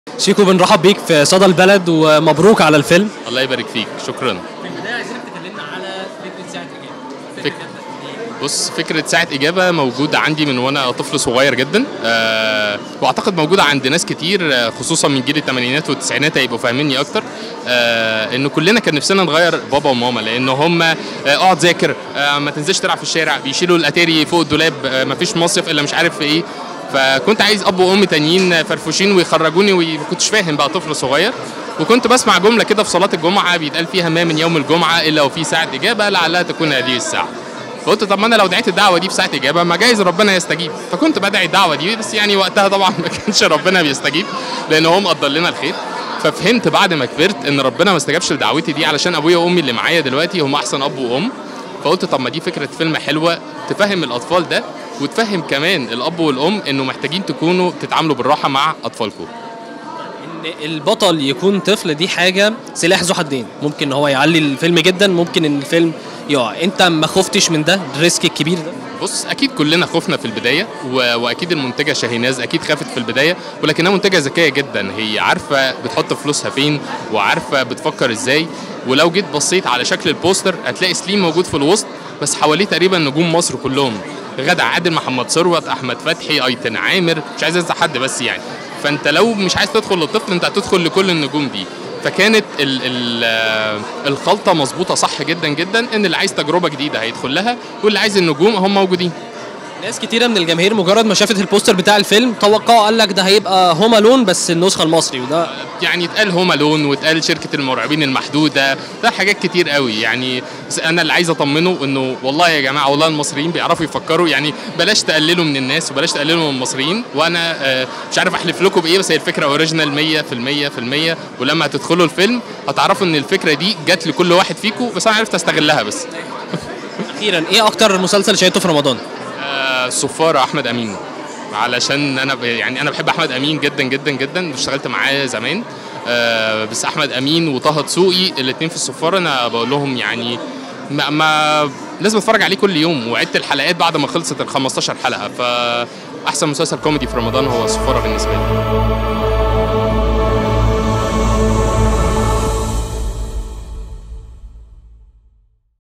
مؤلف ساعة إجابة : فكرة الفيلم مختلفة وبيقدم رسالة للجمهور
ElBaladOfficial#
ElBalad_Official#